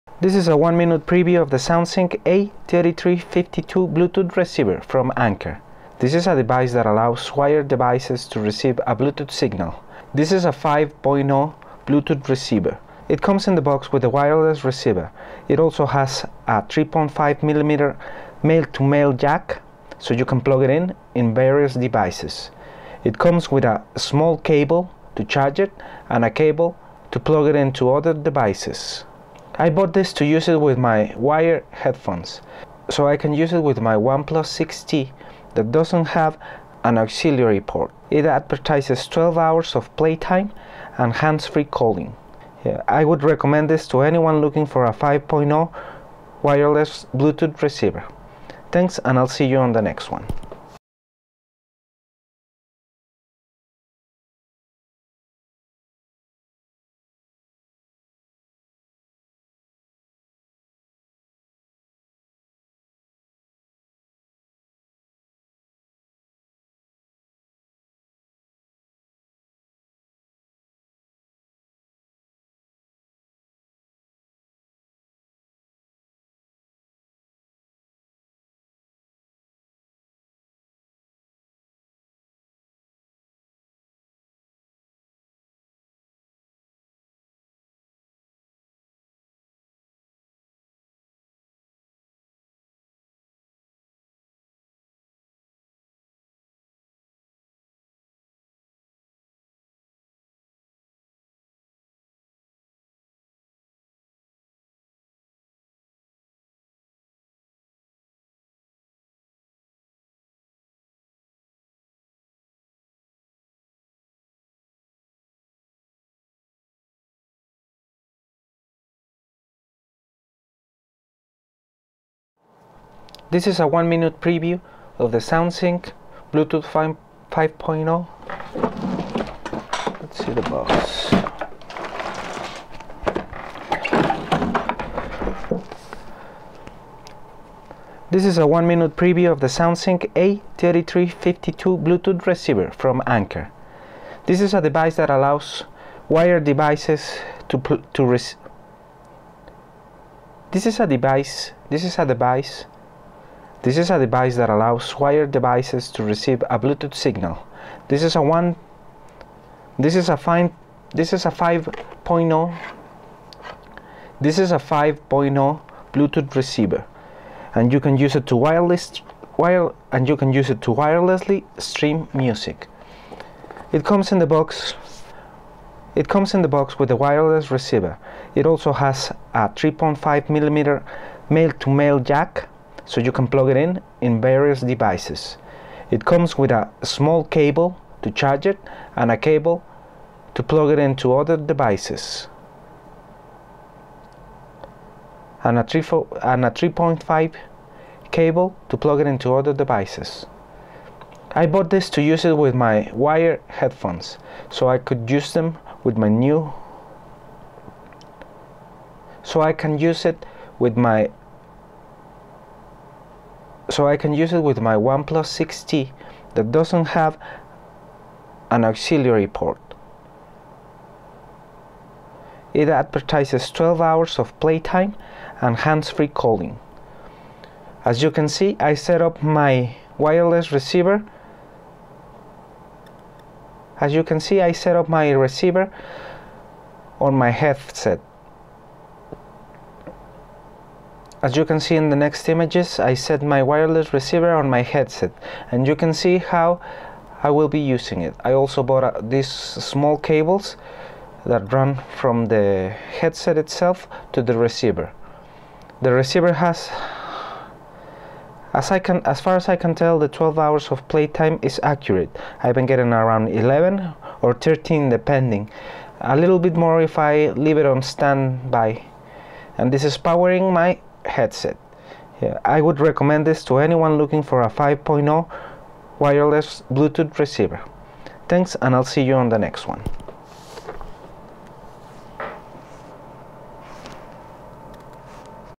One Minute Preview SoundSync Anker Bluetooth 5.0 Receiver
This is a quick one minute preview of this bluetooth reciever that anker makes, it can help you play your song over bluetooth on speakers or wired headphones.